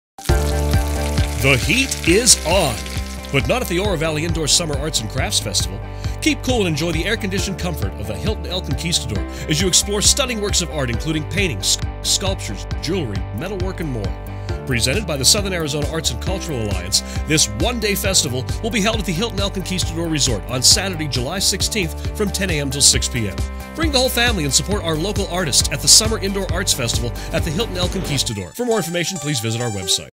Indoor Summer Fine Arts Festival 2016
The festival’s setting at the base of the majestic Pusch View Mountains brings the beautiful desert to life through the many artists inspired by the Southwest’s landscapes and wildlife. The Hilton Indoor Summer Fine Art Festival features up to 50 fine artists from around the region as well as live strolling performances throughout the weekend.